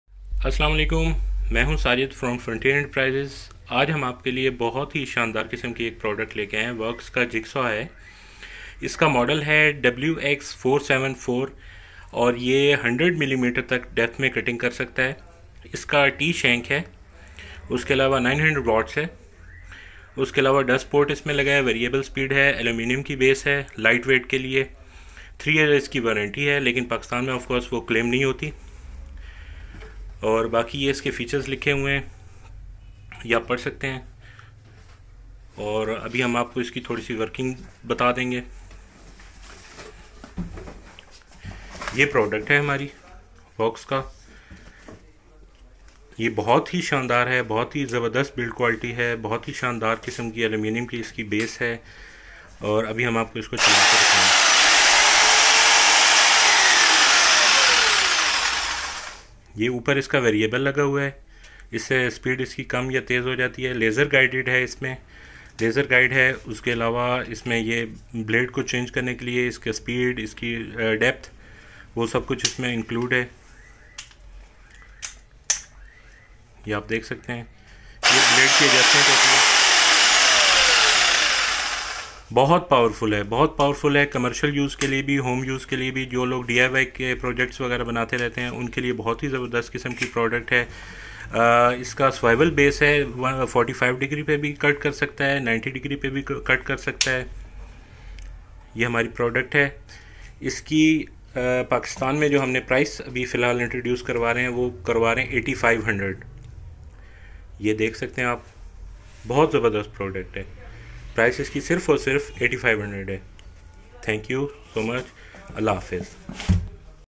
WORX JIGSAW 900 watt unboxing and price by FE
WORX JIGSAW
comercial model
900 watt
best for wood and steel both
212 adamjee road saddar rwp